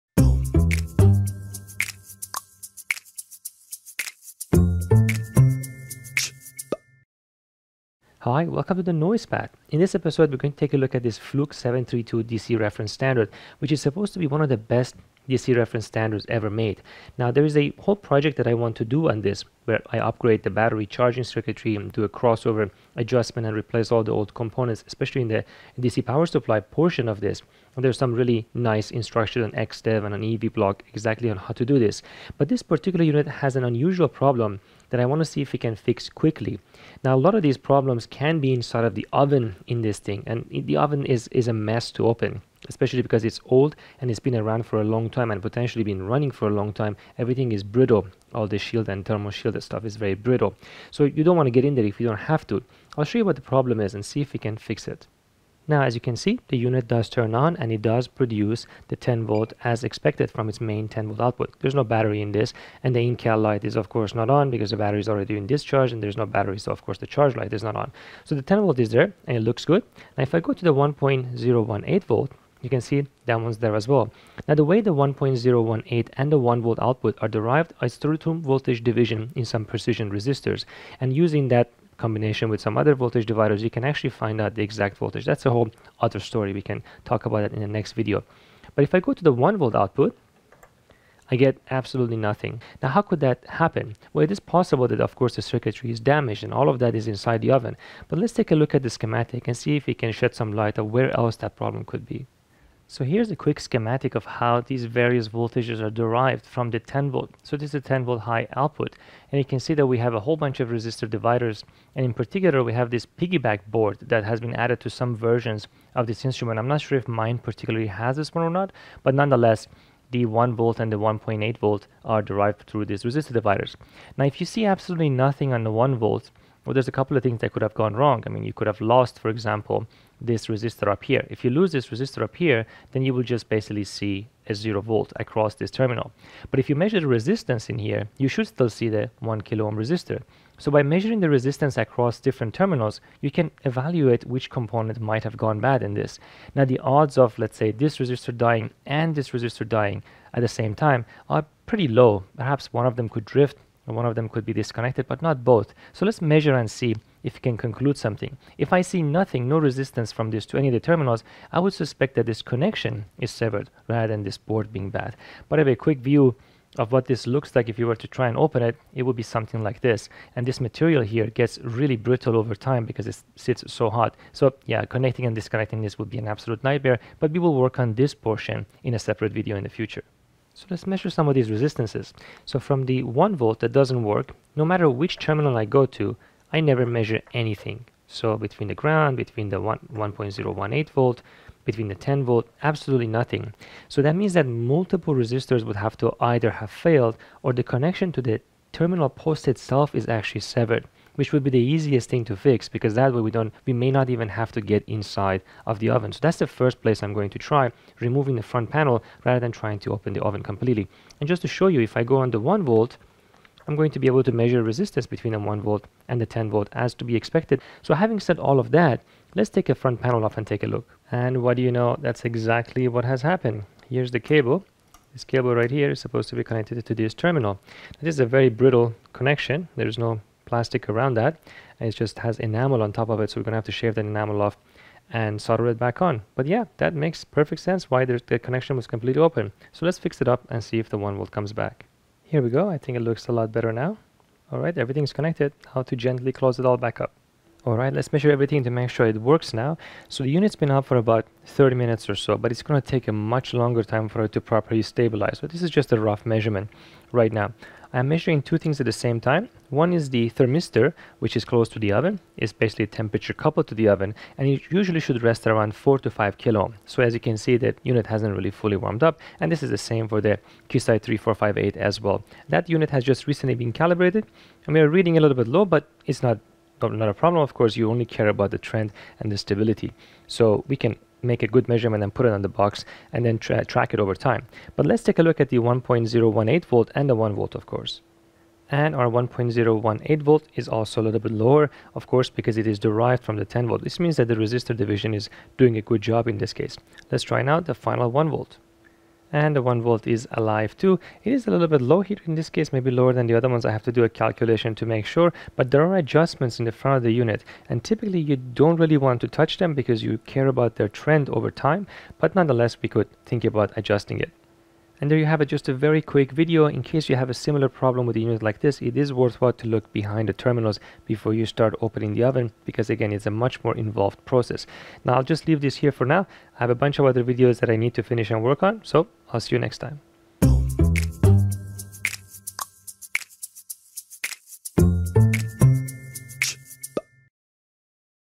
TNP #32 - Fluke 732A DC (10V, 1.018V, 1V) Reference Standard Quick Repair
In this episode Shahriar quickly repairs a faulty Fluke 732A DC Reference Standard. Although the unit does produce a good 10V and 1.018V output, the 1V output shows no voltage at all. A close examination of the schematic shows that the 1V output is derived through a resistor divider from the 10V source.
Since no resistance can be measured from the 1V to any of the other ports, it is suspected that the failure is caused by a broken connection behind the front panel. The connection is corrected and the unit produces all desired output signals.